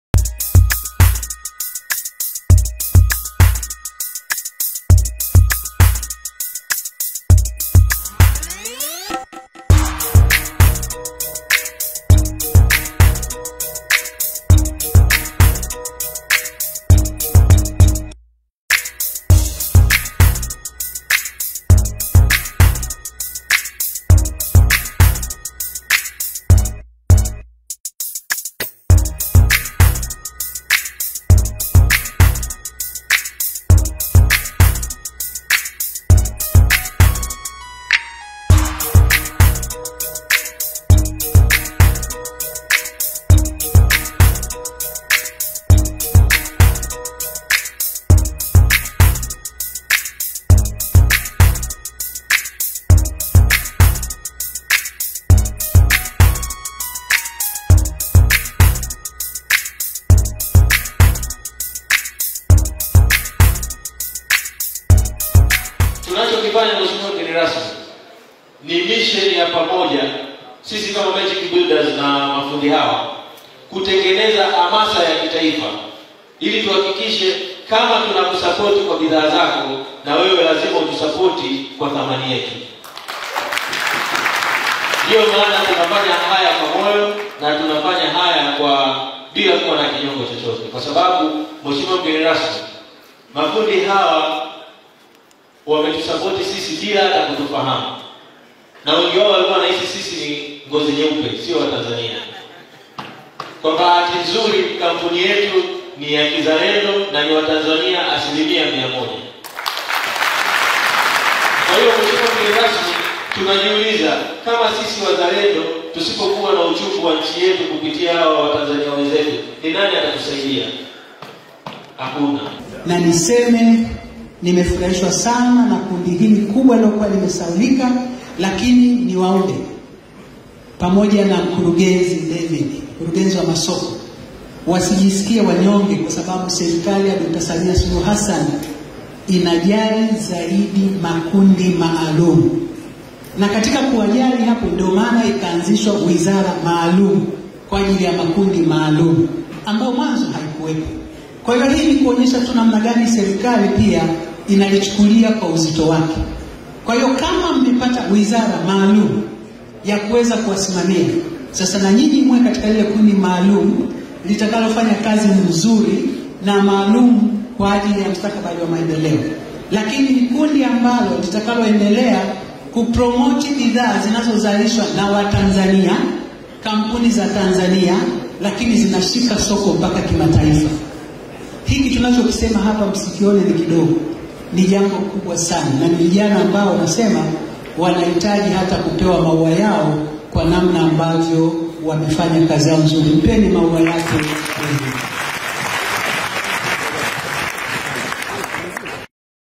Mafundi Rangi Mwanza wakutanishwa na Magic Builders International Ltd, Washinda Pikipiki
Kampuni ya Magic Builders International Ltd, kama kawaida jana waliandaa Semina ya Mafundi Ujenzi Jijini Mwanza iliyofanyika katika ukumbi wa Kwa Tunza na Mafundi Takriban 15000 walihudhuria katika Semina hiyo. Mbunge wa jimbo la Ilemela Mhe. Angeline Mabula ndiye alikuwa Mgeni rasmi na alitoa vyeti vya semina hiyo kwa Mafundi pamoja na kugawa zawadi za Simu kwa Mafundi walijibu kwa ufasaha maswali ya Bidhaa zinazozalishwa na Magic Builders International Ltd na Pikiki kwa mafundi walioshida kwa kupakua coupon nyingi zaidi zinazopatikana kwenye kila mfuko wa whiteskim wallputty.